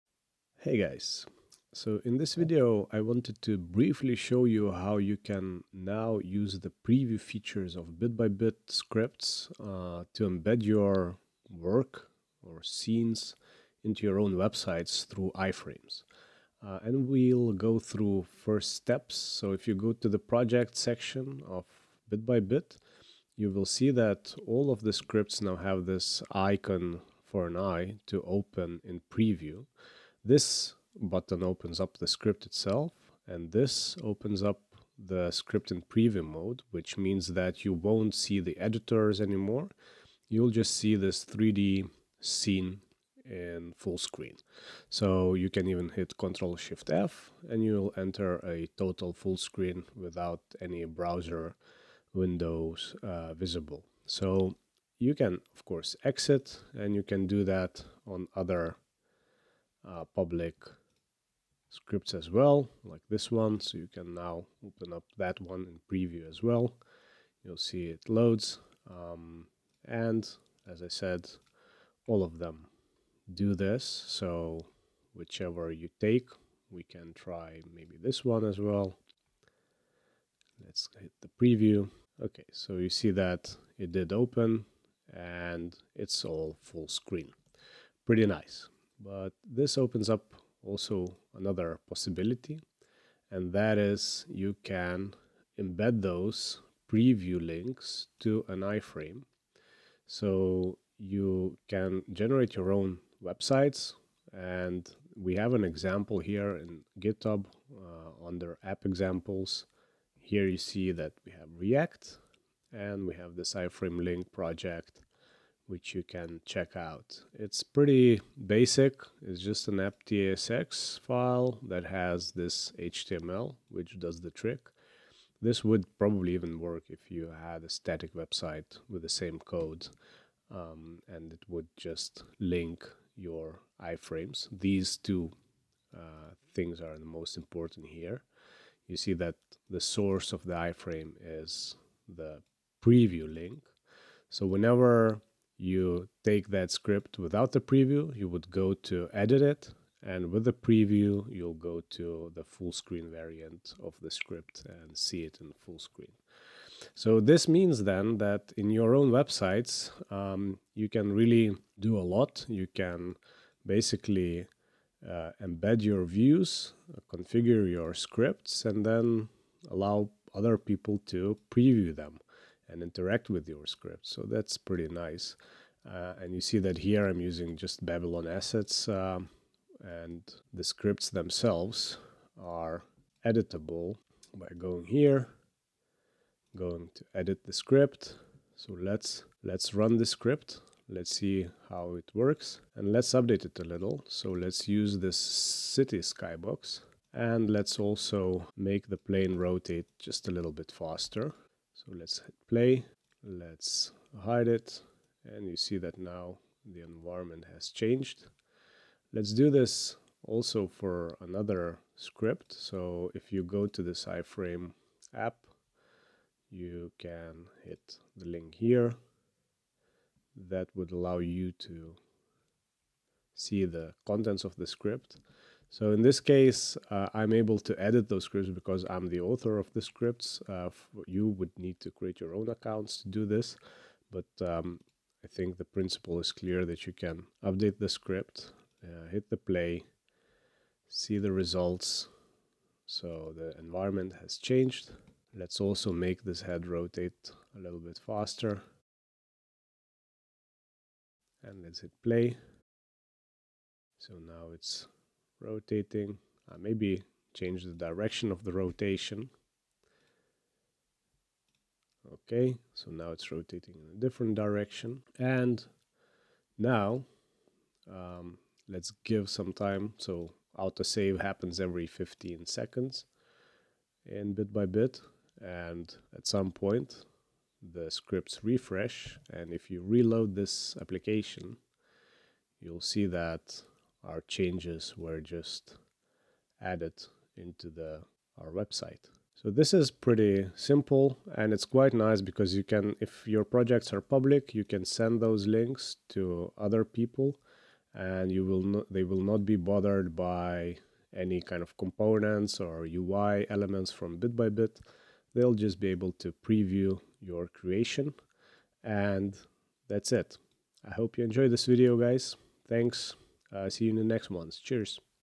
Preview feature and IFrame embeds
Now you can use our new PREVIEW links of the scripts to share full screen views of your public 3D scenes with other people. This new feature also allows you to embed iframe views into your own websites in order to show off your 3D designs. Keep in mind that for some interactions with an iframe you will need to use a special permission attributes.